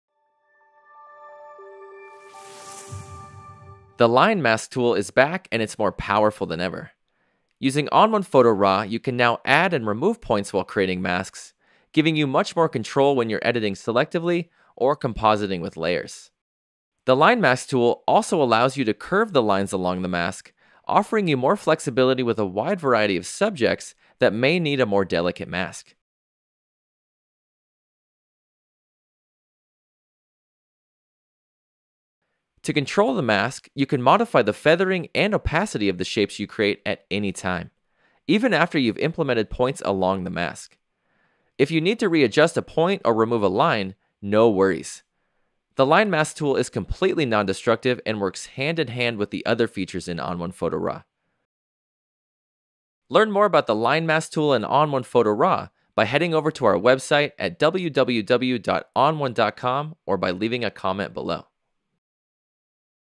The Line Mask Tool is Back
The line mask tool is back, and it's more powerful than ever. Using ON1 Photo RAW 2022, you can now add and remove points and easily curve lines for creating masks, giving you much more control when you're editing selectively or compositing with layers.